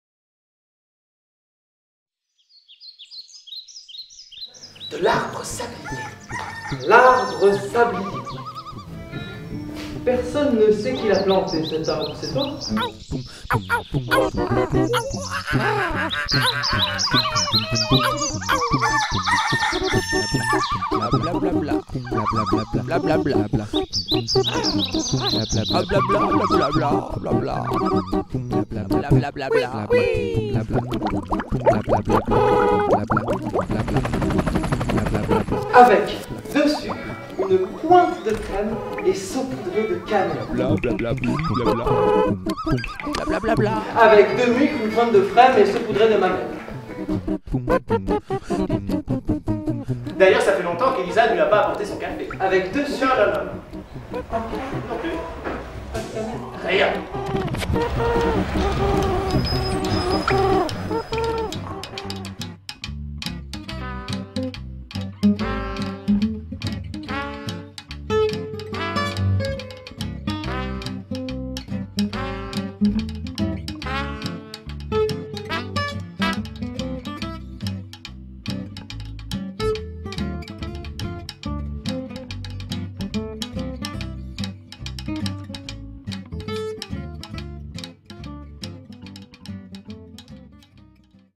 L' Arbre Sablier
Spectacle tout public, créé par le "Collectif Mirificus".
Captation et Montage du teaser, Caroline ORSINI.